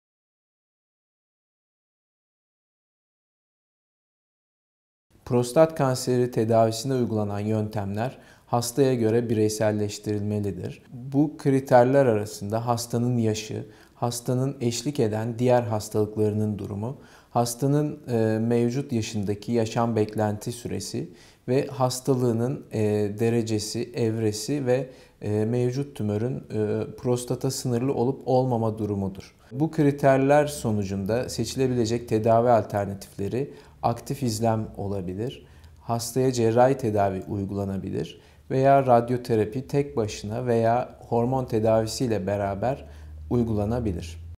Prostat kanseri tedavisinde uygulanan yöntemler nelerdir?
Acıbadem Maslak Hastanesi, Üroloji Uzmanı Yrd. Doç. Dr. Selçuk Keskin, Prostat Kanseri üzerine soruları yanıtlıyor;